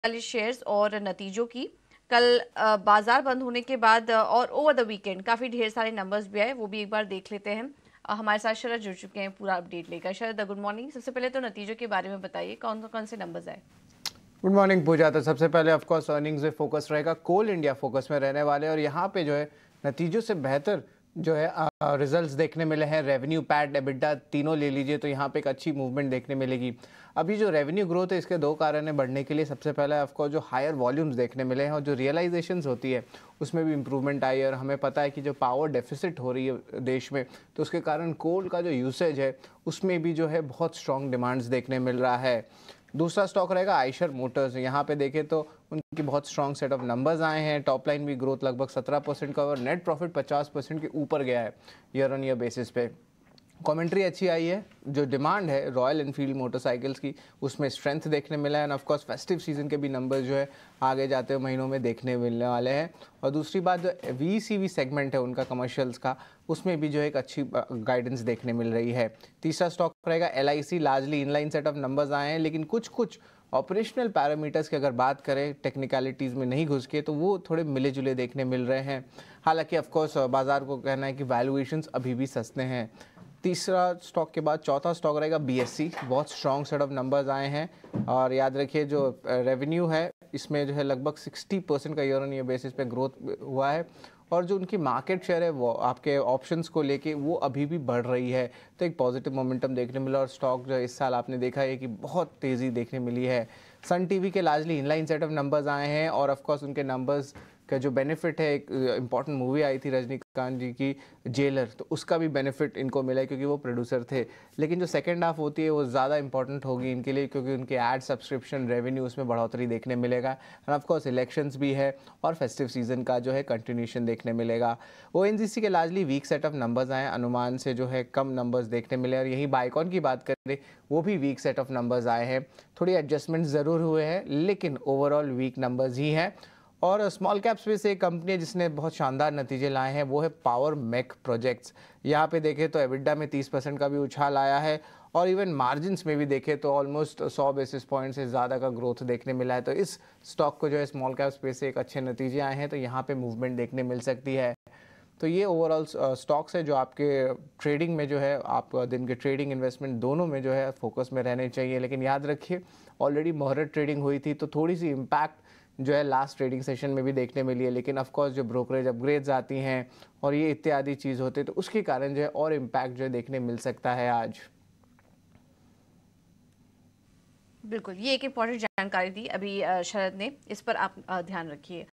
Coal India, LIC, ONGC समेत किन कंपनियों के आए Q2 Results? नतीजों से फोकस में रहेंगे ये Top Stock
Coal India, LIC, Eicher Motors, BSE, Son TV, ONGC, Power Make Products Limited Share में जानें निवेश को लेकर क्या है Experts की राय. जानें किन levels पर करनी चाहिए आपको खरीदारी और क्या है Target Price और जानिए किस Strategy के साथ इस Stock में पैसा कमाया जा सकता है. देखें वीडियो.